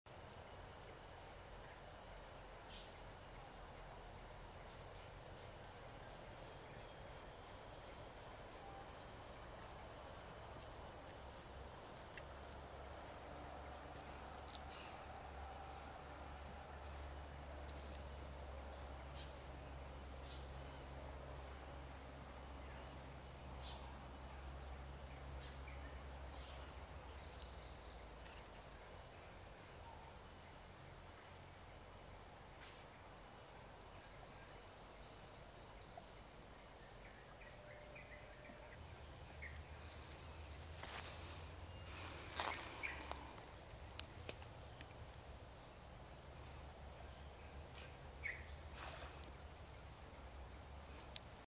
A pond teeming with dragonflies (Please watch in HD quality)
A pond teeming with dragonflies in Singapore.

Can you see Hydrobasileus croceus ovipositing with contact guarding? As far as I know, in Singapore, H. croceus is the only libellulid which exhibits contact guarding in flight.